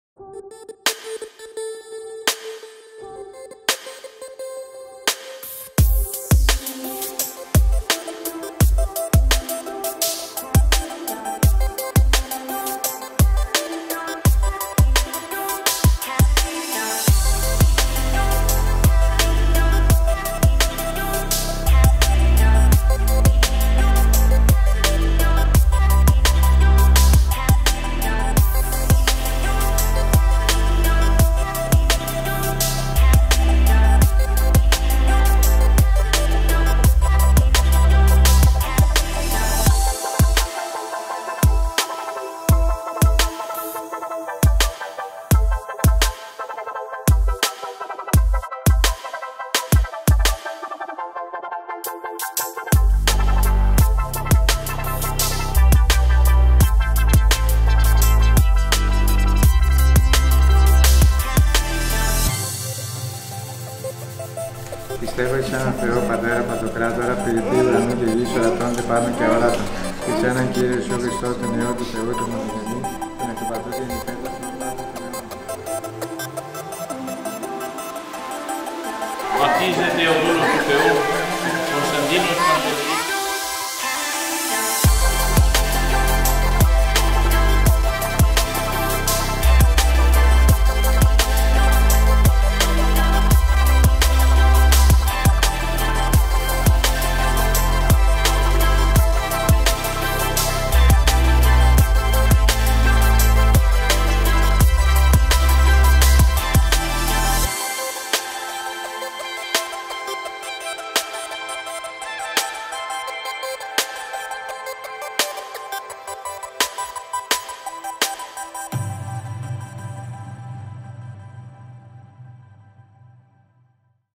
Baptism St George Kavouri
Βίντεο Βάπτισης στον Άγιο Γεώργιο στο Καβούρι.
Από τα πιο ιδιαίτερα και γραφικά ξωκλήσια ο Άγιος Γεώργιος στα νότια προάστια της Αττικής. Βρίσκεται σε ένα πολύ φιλόξενο και ευρύχωρο παρκάκι με ψηλά πεύκα. Διαθέτει εύκολο πάρκινγκ και άνετο χώρο για το κέρασμα. Πολύ κοντά βρίσκονται διάφοροι χώροι για τη δεξίωση της βάπτισης.

Για τη φωτογράφιση και το βίντεο της βάπτισης ή του γάμου σας,

Για να μην χάνεις τα νέα μας βίντεο και τις προσφορές μας